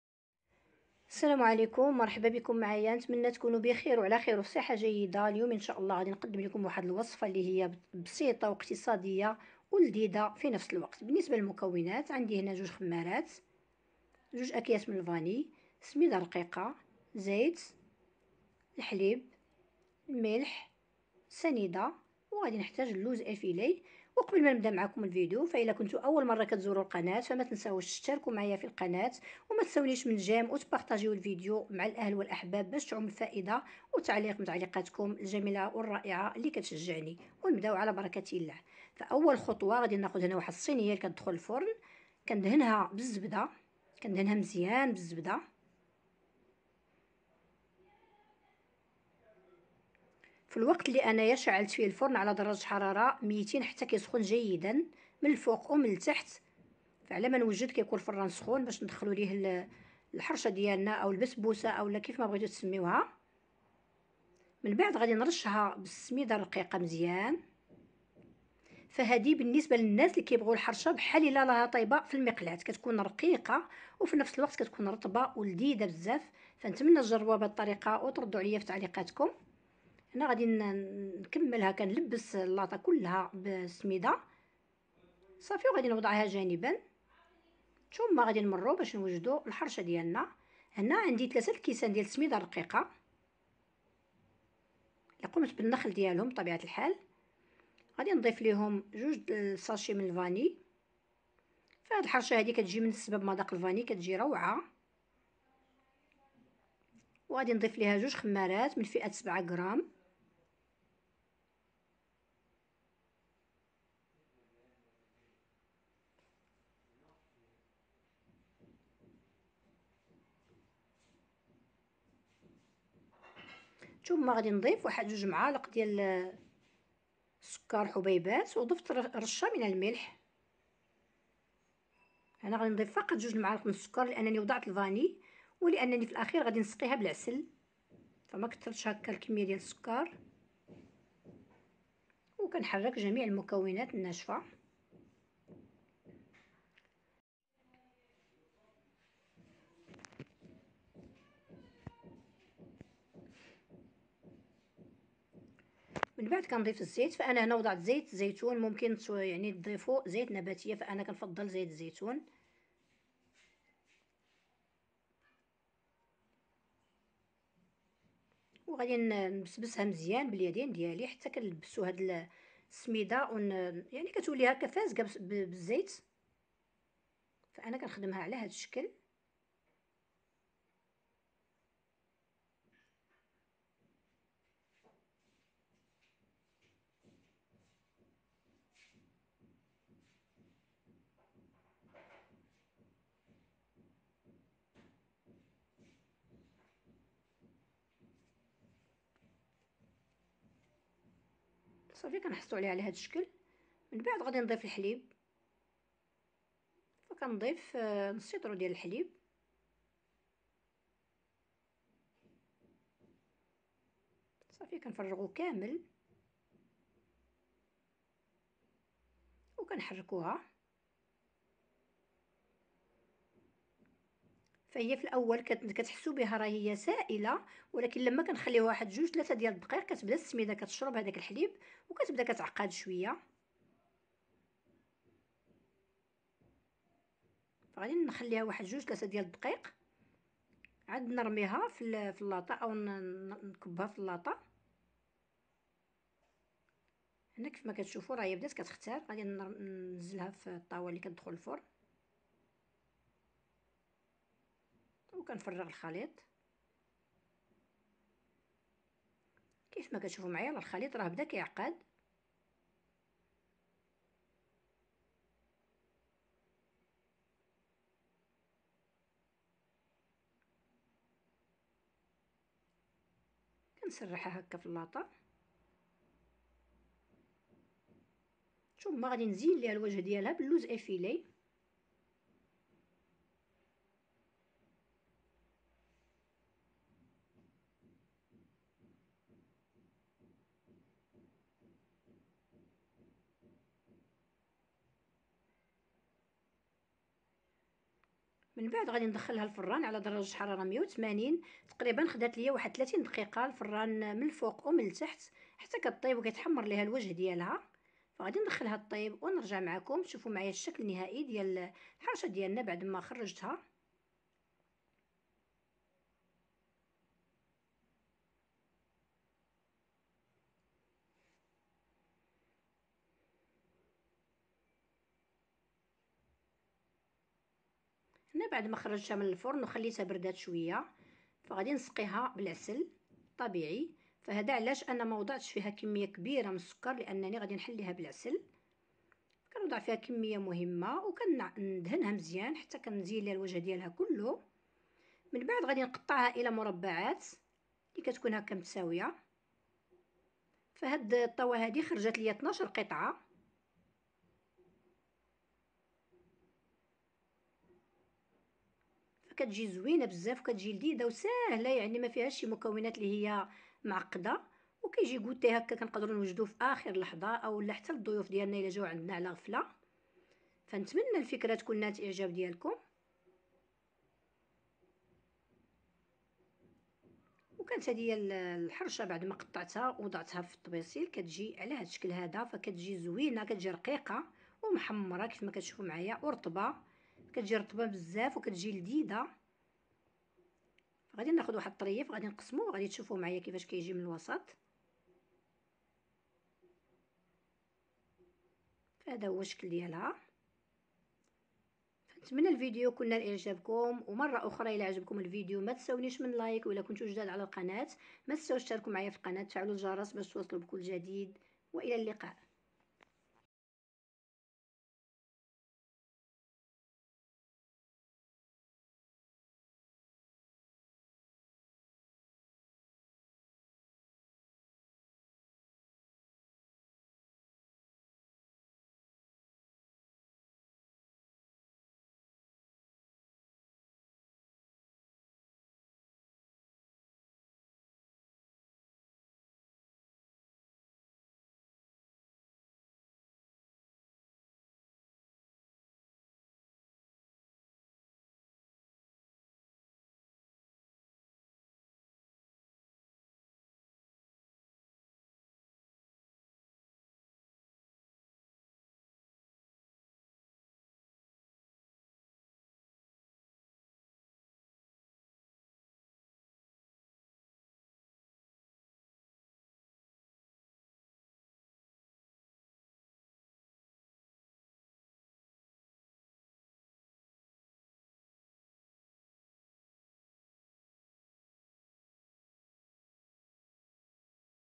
أسرع وأسهل فطور بنصف لتر حليب😋😋😋
المقادير

1/2لتر حليب
4ملاعق سكر
3كؤوس سميد ناعم
القليل من الملح
2خمارات من فئة 7غرام
1/2كأس من الزيت
2أكياس من الفنيلا